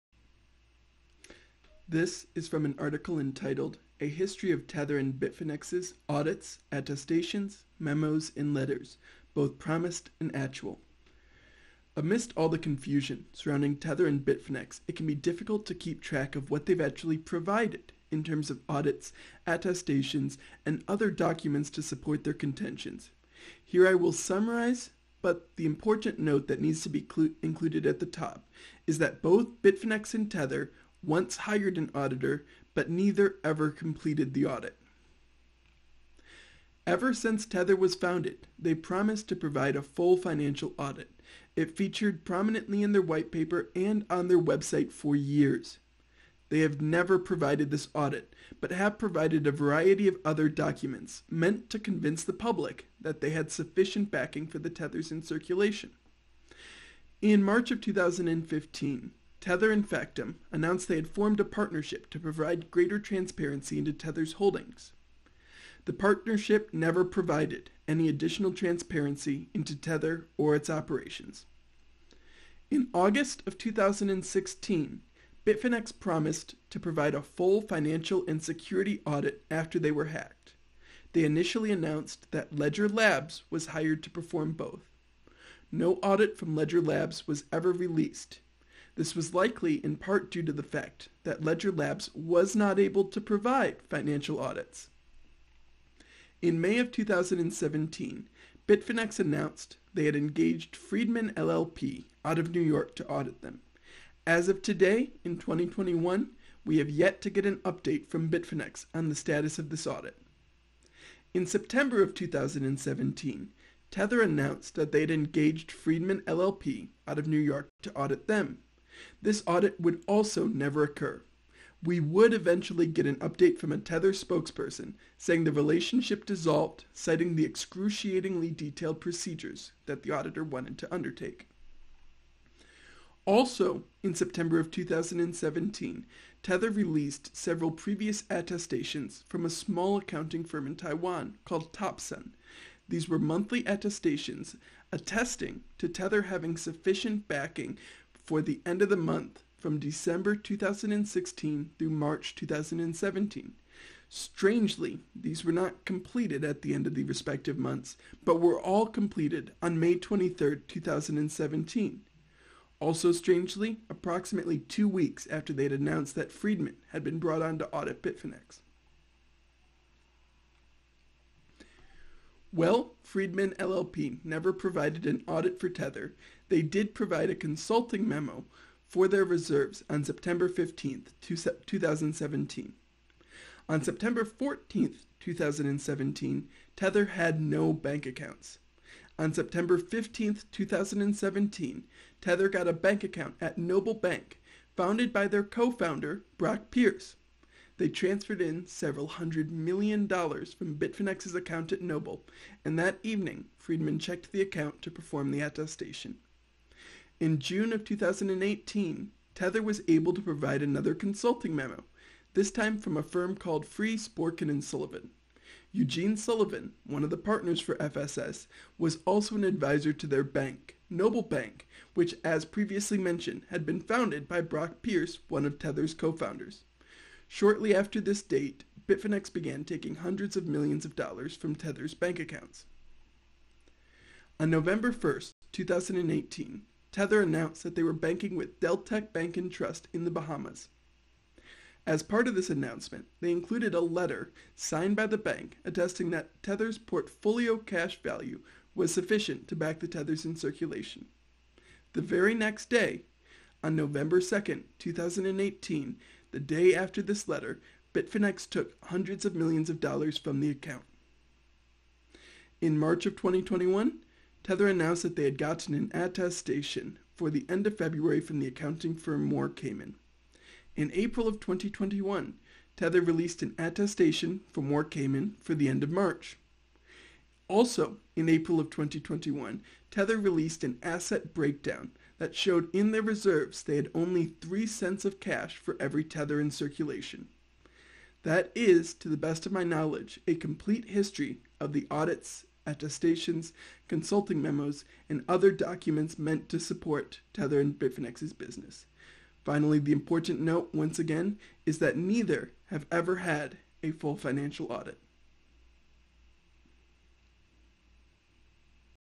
A History of Tether and Bitfinex’s Audits, Attestations, Memos, and Letters: Promised and Actual
Amidst all the confusion surrounding Tether and Bitfinex it can be difficult to keep track of what they have actually provided in terms of audits, attestations, and other documents to support their contentions. Here I will summarize, but the important note that needs to be included at the top is that both Bitfinex and Tether once hired an auditor, but neither ever completed the audit.

Ever since Tether was founded they have promised to provide a full financial audit. It feature prominently in their whitepaper and on their website for years. They have never provided this audit, but have provided a variety of other documents meant to convince the public that they had sufficient backing for the tethers in circulation.

In March of 2015 Tether and Factom announced (archive) they had formed a partnership to provide greater transparency into Tether’s holdings. The partnership never provided any additional transparency into Tether or its operations.

In August of 2016 Bitfinex promised (archive) to provide a full financial and security audit after they were hacked. They initially announced that Ledger Labs was hired to perform both. No audit from Ledger Labs was ever released. This was likely in part due to the fact that Ledger Labs was not able to provide financial audits.

In May of 2017 Bitfinex announced (archive) they had engaged Friedman LLP out of New York to audit them. As of today in 2021 we have yet to get an update from Bitfinex on the status of this audit.

In September of 2017 Tether announced (archive) that they had engaged Friedman LLP out of New York to audit them. This audit would never occur, we would eventually get an update from a Tether spokesperson saying the relationship dissolved, citing the “excruciatingly detailed procedures” (archive) that the auditor wanted to undertake.

Also in September of 2017 Tether released from previous attestations from a small accounting firm in Taiwan called TOPSUN. These were monthly attestations attesting to Tether having sufficient backing for the end of the month from December 2016 through March 2017. Strangely these were not completed at the end of the respective months but were all completed on May 23rd 2017. (Strangely approximately two weeks after they announced that Friedman had been brought on to audit Bitfinex)

Friedman LLP never provided an audit for Tether, but they did provide a consulting memo (archive) (my copy) to their reserves on September 15th 2017. On September 14th 2017 Tether had no bank accounts. On September 15th 2017 Tether got a bank account at Noble Bank, founded by their co-founder Brock Pierce. They transferred in several hundred million dollars from Bitfinex’s account at Noble and that evening Friedman checked the account to perform the attestation.

In June of 2018 Tether was able to provide another consulting memo (archive) (my copy), this time from a firm called Freeh, Sporkin & Sullivan. Eugene Sullivan one of the partners for FSS was also an advisor to their bank, Noble Bank, which had been founded by Brock Pierce, one of Tether’s cofounders. Shortly after this date Bitfinex began taking hundreds of millions from Tether’s bank accounts.

On November 1st 2018 Tether announced they were banking with Deltec Bank and Trust in the Bahamas. As part of this announcement they included a letter (archive) (my copy) signed by the bank attesting that Tether’s ‘portfolio cash value’ was sufficient to back the tethers in circulation. On November 2nd 2018, the day after this letter Bitfinex took hundreds of millions of dollars from the account.

In March of 2021 Tether announced they had gotten an attestation (archive) (my copy) (article on it) for the end of February from the auditing firm Moore Cayman.

In April of 2021 Tether released an attestation (archive) (my copy) from Moore Cayman for the end of March.

In April of 2021 Tether released an asset breakdown (archive) (my copy), these two pie charts showed they had only $0.03 in cash for every Tether.